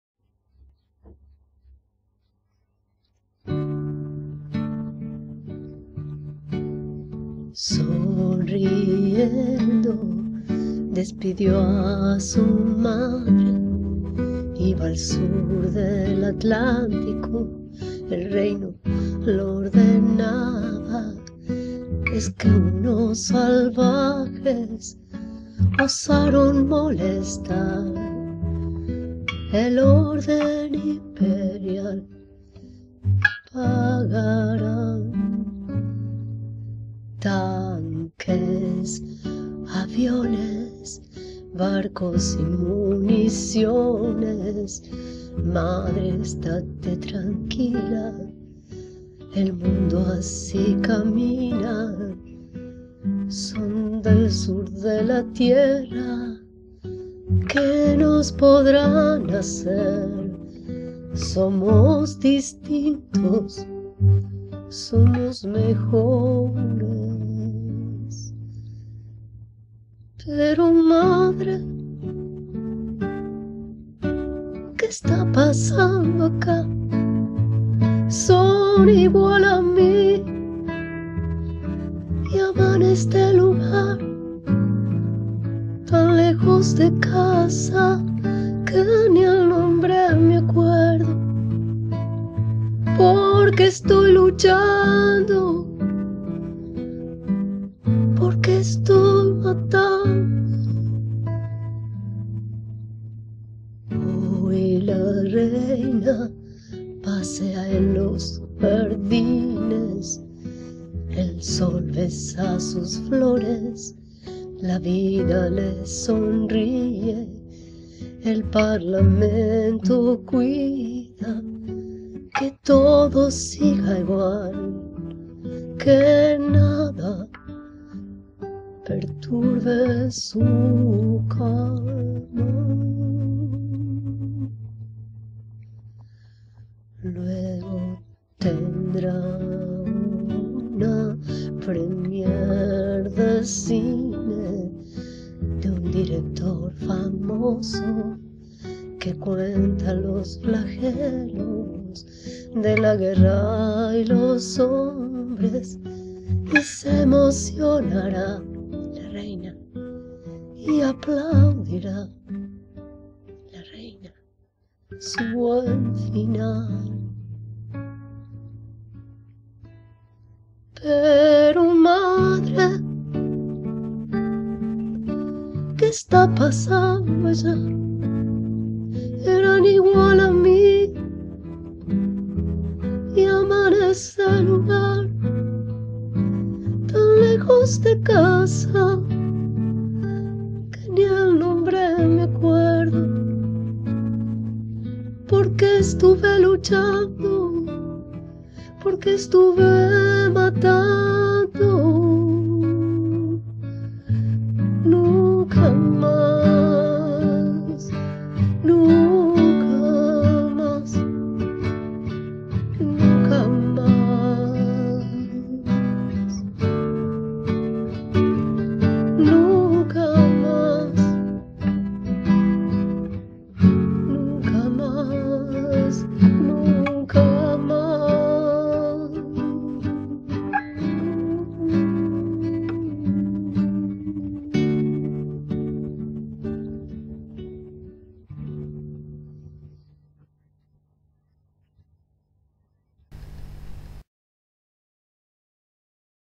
Homenaje a los Héroes de Malvinas por cris buscaglia lenz
Diseño de video,guitarra y voz por cris buscaglia lenz
Esta canción,de Raúl Porchetto,va dedicada a todas las personas que sufren por la locura de las guerras.